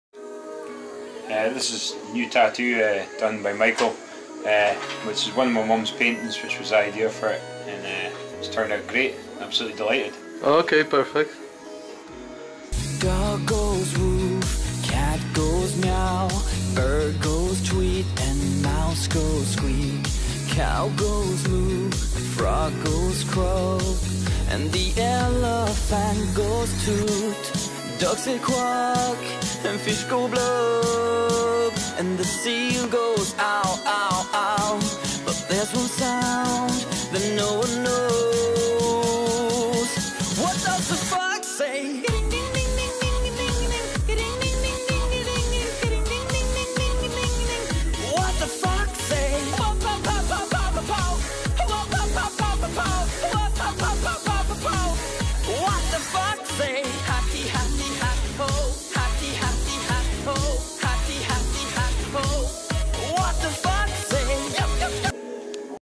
Watercolor Tattoo Michael da Bear
New watercolor tattoo done by Michael.
Inspired by Customer's mum painting.
Start of full sleeve in this incredible style ;)